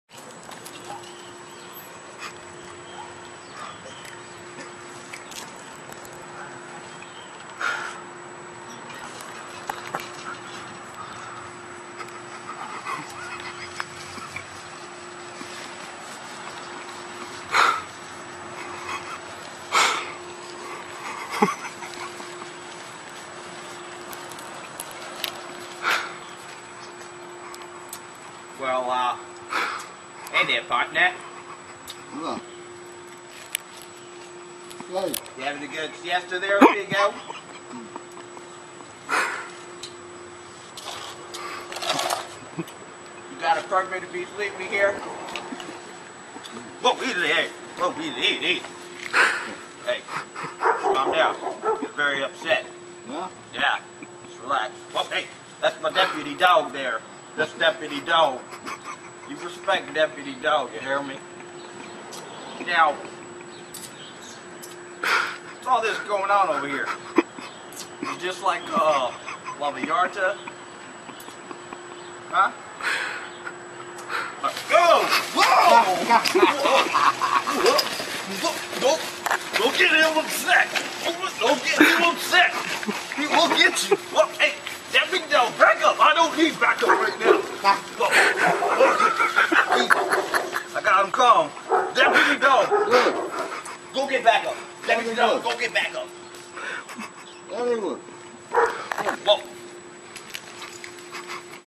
Deputy Dogg
before anyone starts to fuzz. I know him hes a friendly wineo that walks my alley everyday and messes with my dogs everyday, but on this day we mess with him. he woke from his drunken nap and we all had a good laugh. No humans or dogs were hurt in the making of this video.